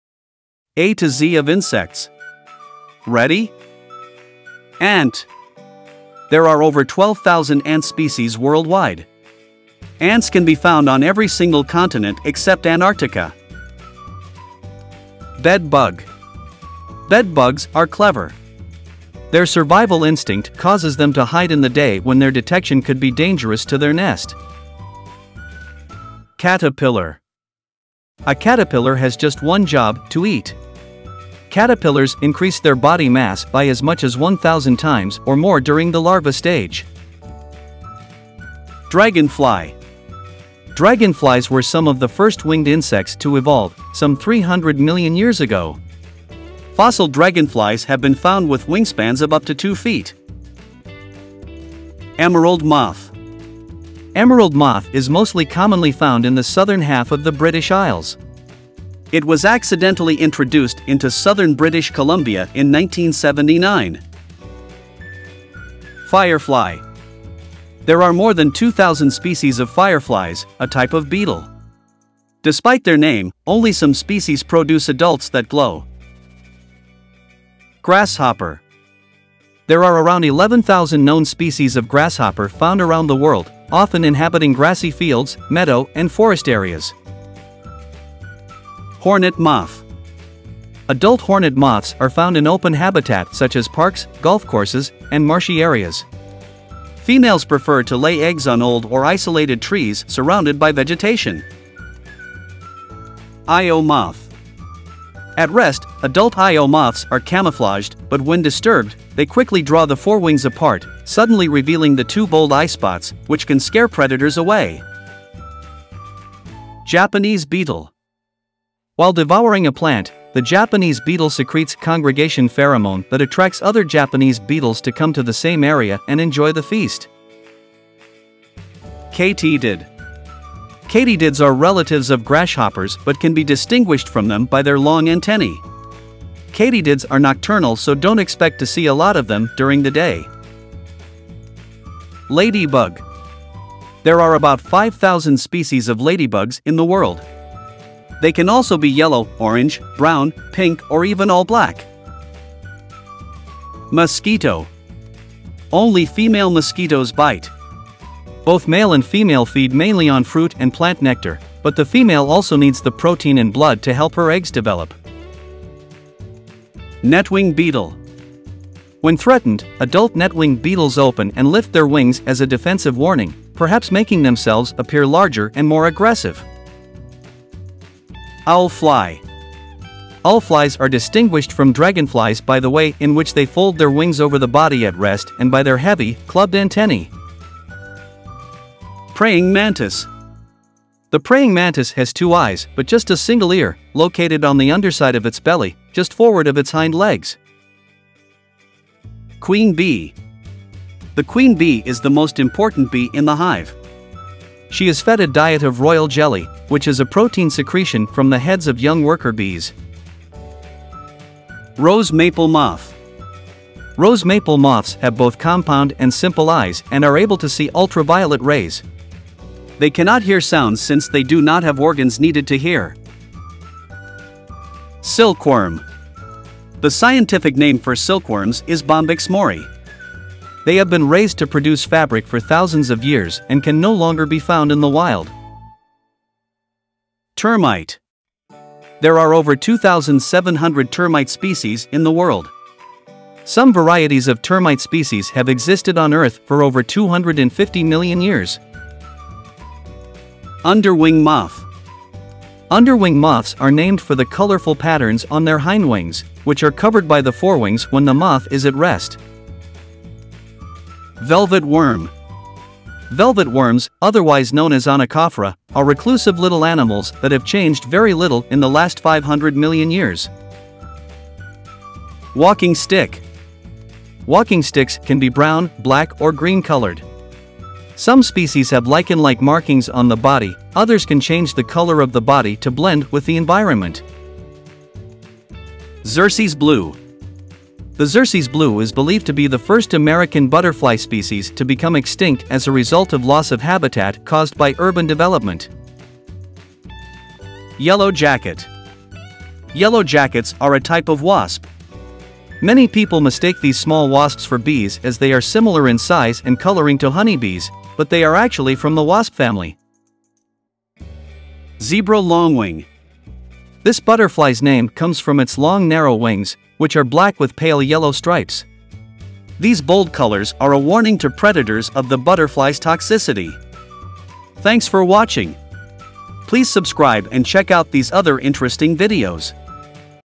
A to Z Insects, ABC Insects, Alphabet Insects | Insect Names & Fun Facts
AtoZ World brings to you the A to Z of Insects, the ABC of Insects.
Do you know Insect names for each letter of the English Alphabet? Is there an Insect name for each letter of the English Alphabet? Watch the video to find out!

To keep things simple, only one entry is allowed for each letter. So, did your choices for each letter make it to the list? Did I miss a popular entry?

IMAGES
All the names and images used in this video are for information, entertainment, and review purpose only.